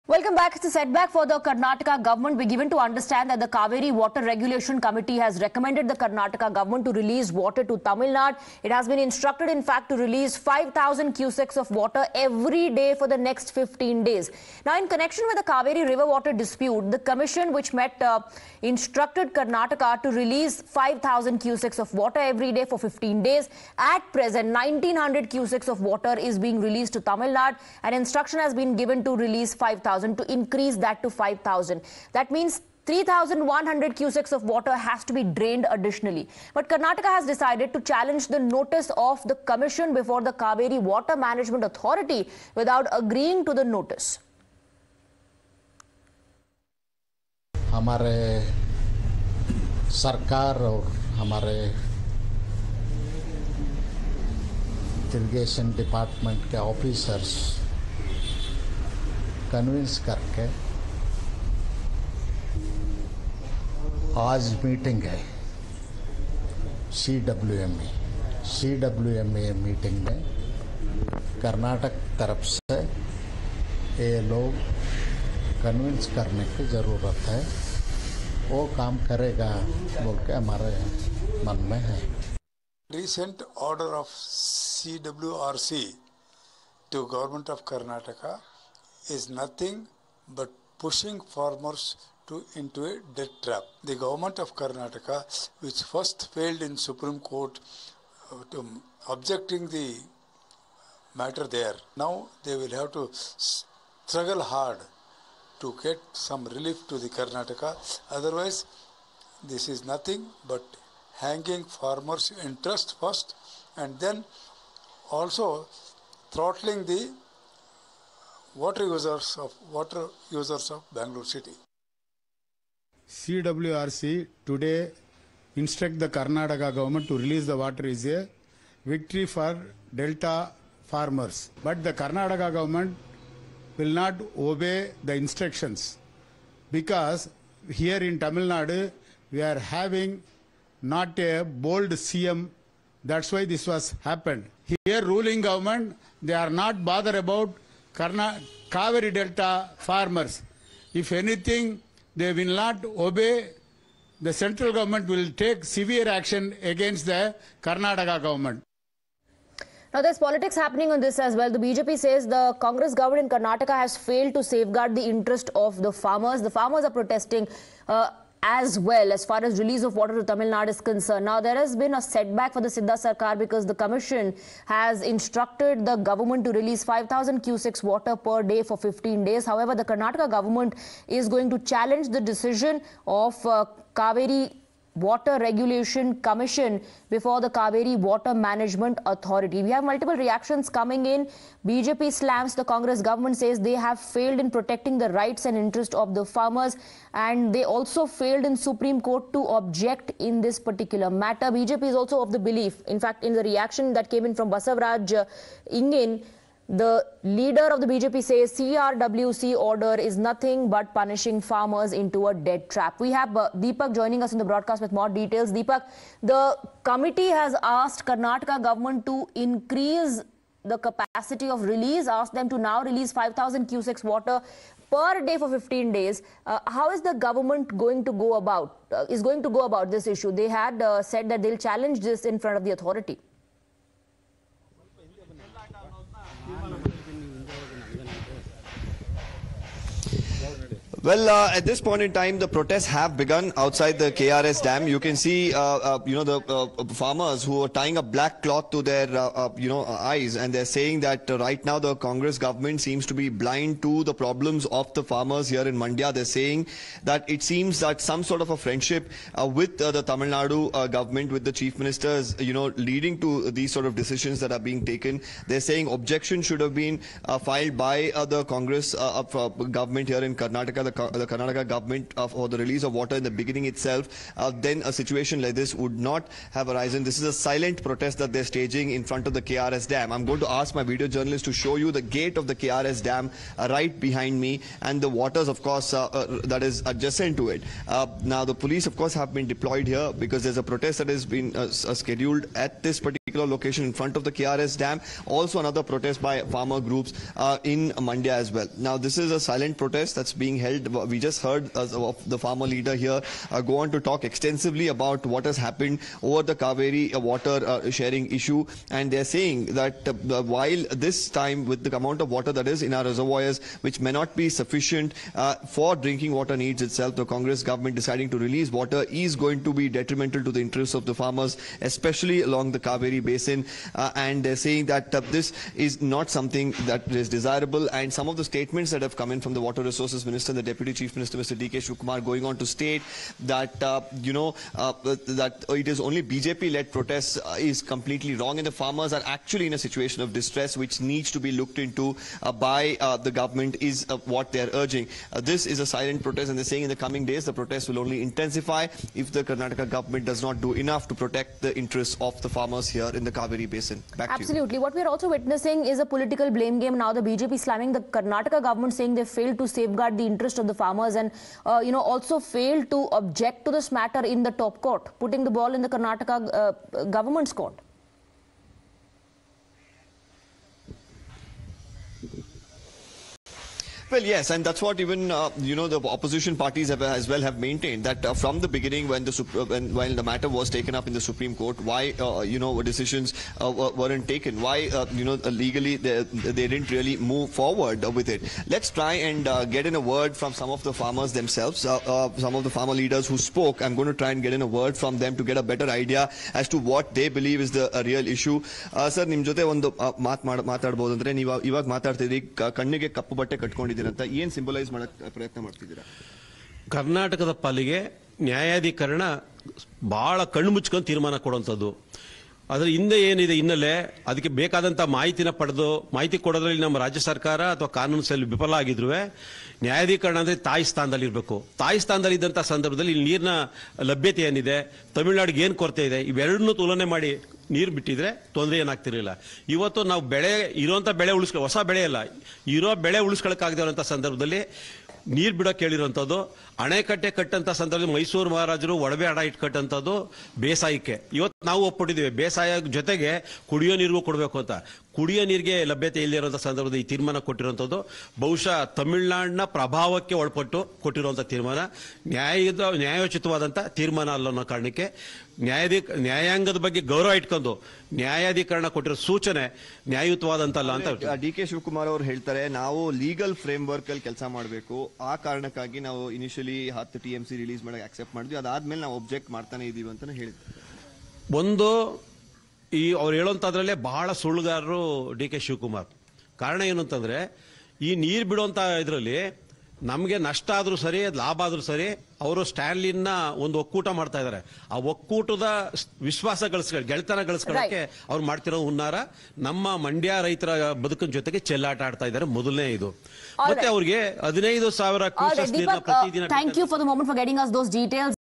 CWRC Suggests Karnataka To Release 5,000 Cusecs Of Cauvery Water To Tamil Nadu For Period Of 15 Days
It's a setback for the Karnataka government, as the Cauvery water regulation committee has recommended the Karnataka government to release water to Tamil Nadu. It has been instructed to release 5000 cusecs of water every day for the next 15 days. At present, 1900 Cusecs of water is being released to Tamil Nadu, and instructions have been given to increase that to 5000 Cusecs, which means 3100 Cusecs of water have to be drained additionally. Karnataka Govt has decided to challenge the notice of the commission before the Cauvery Water Authority without agreeing to the notice. Watch the entire video to know more.